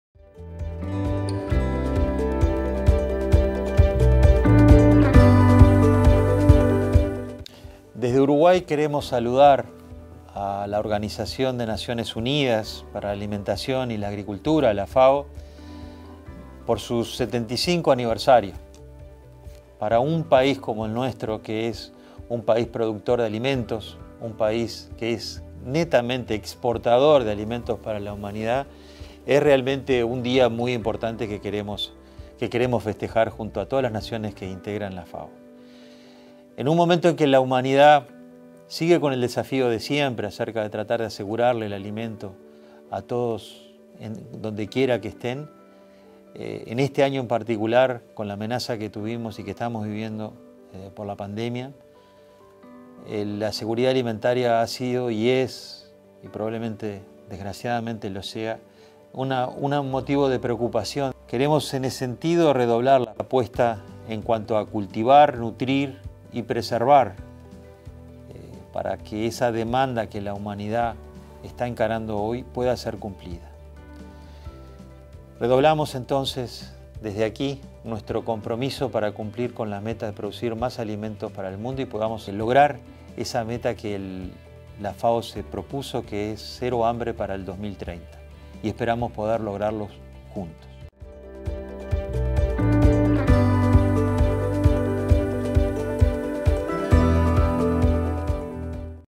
Ministerio de Ganadería Agricultura y Pesca de Uruguay en el Día Mundial de la Alimentación
Mensaje de  Carlos María Uriarte, Ministro de Ganadería Agricultura y Pesca de Uruguay, con motivo del Día Mundial de la Alimentación.